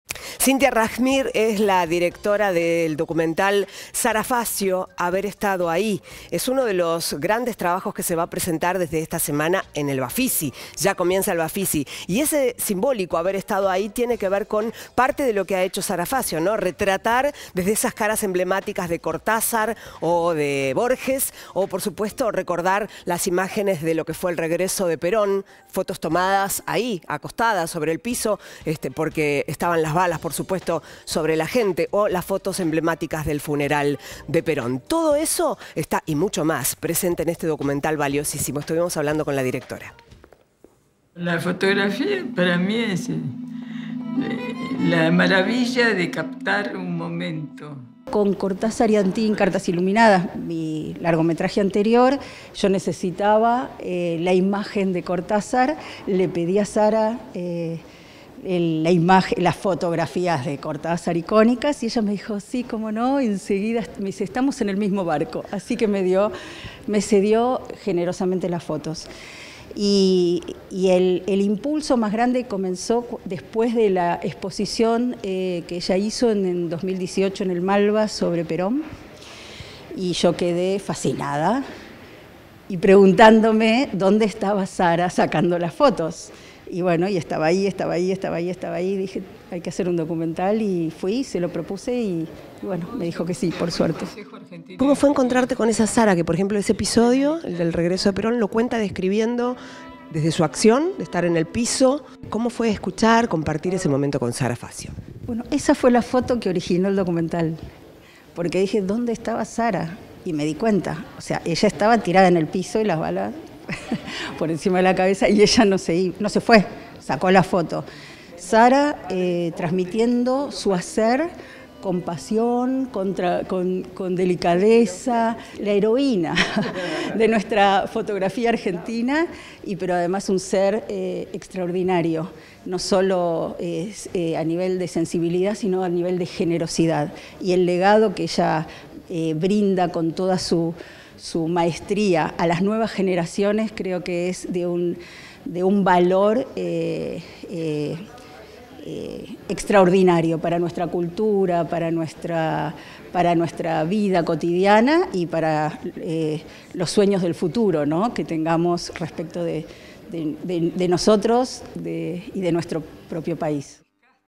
Presentan "Sara Facio: haber estado ahí", en el BAFICI
La película de Cinthia Rajschmir tendrá varias funciones disponibles durante la nueva edición del BAFICI, que se llevará a cabo del 19 de abril al 1 de mayo.